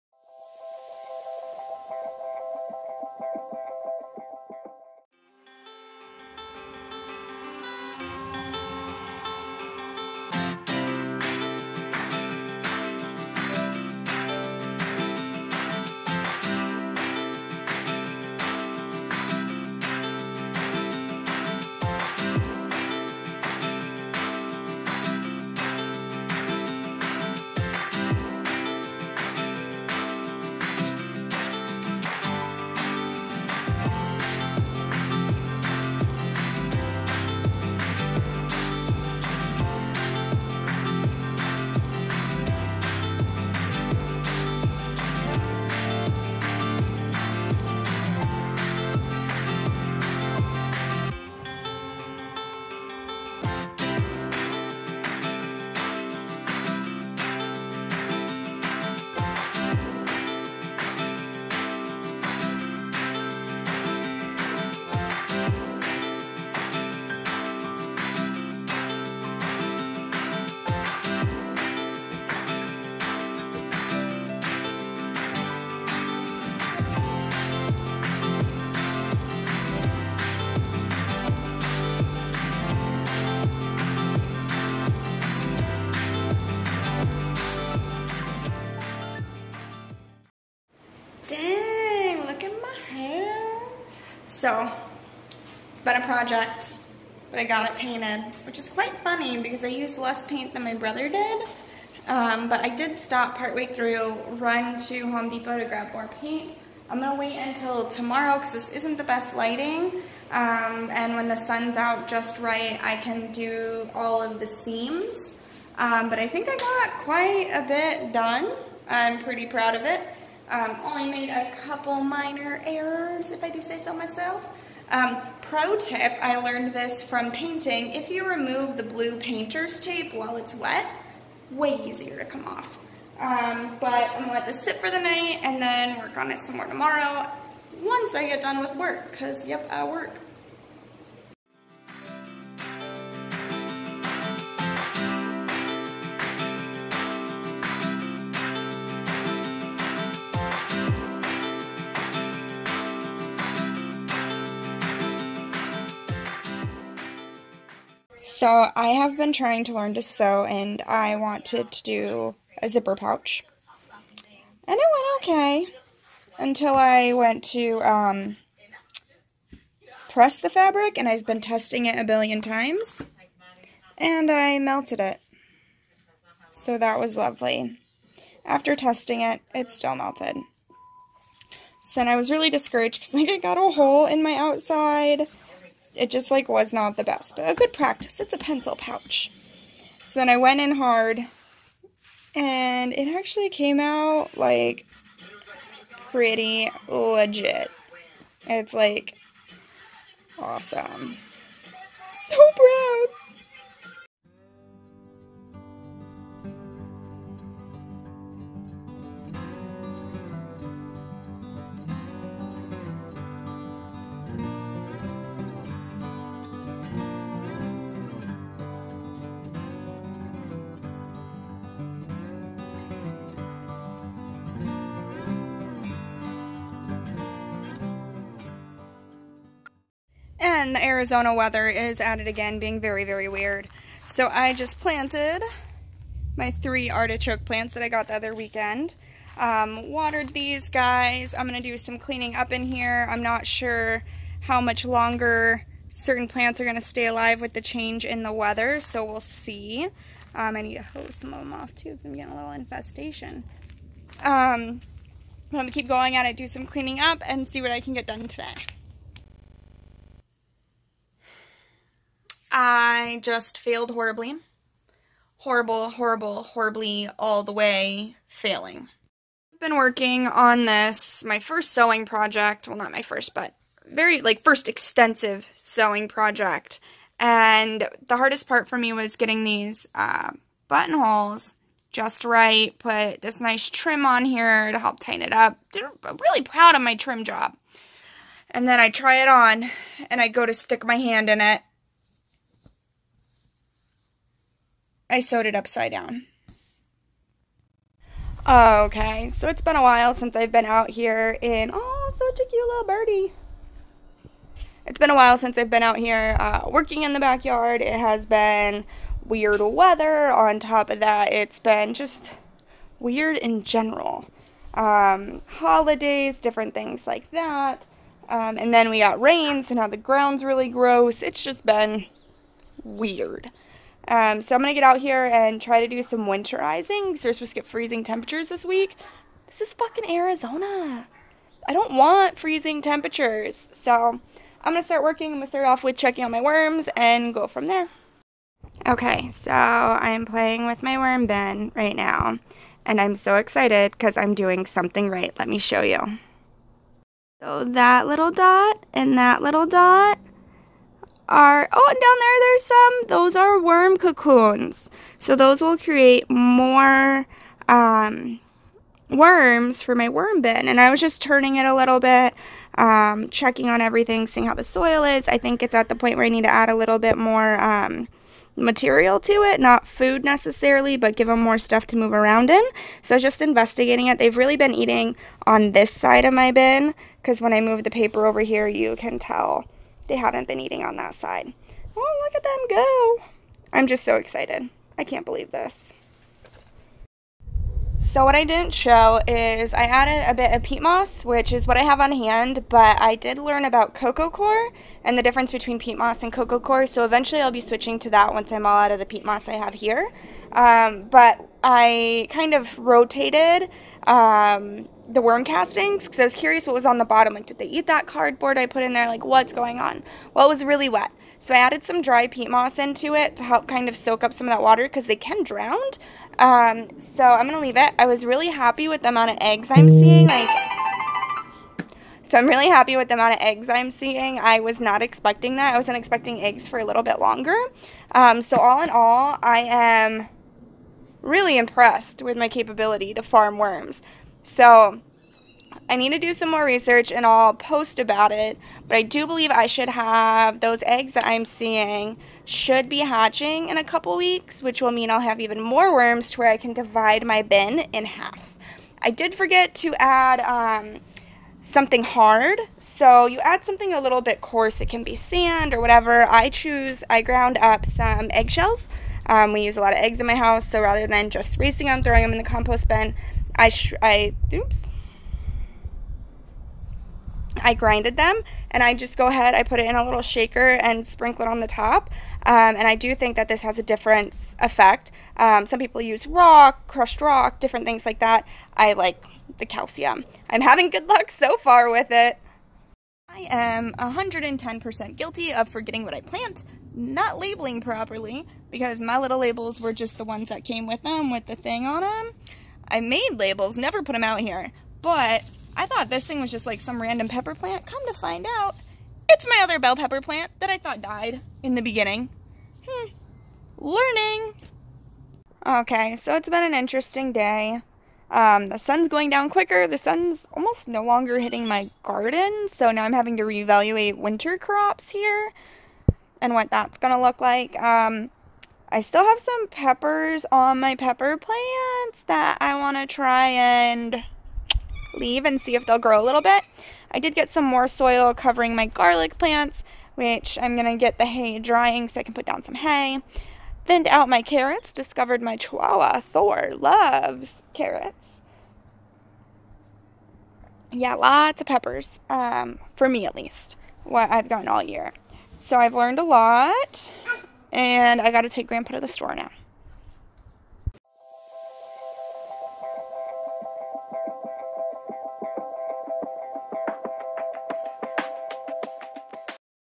What have I done, and why haven't I posted?!?
Well to say the least November has been crazy. My external drive crashed and I lost a lot of footage that I thought was backed up. On top of that  my computer broke. I could not get a break with completing videos.

Yet, life continues. I am working in the garden, playing with worms, and papa is keeping me on my toes.